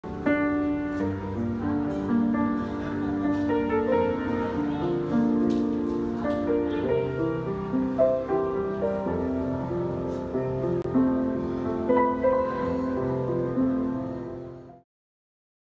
Teaser Trailer in LivingMalaysia: A Pianist from Qatar
A Pianist in Qatar: Hossam Hassan Quenches a Desert with his Brand of Fluid Music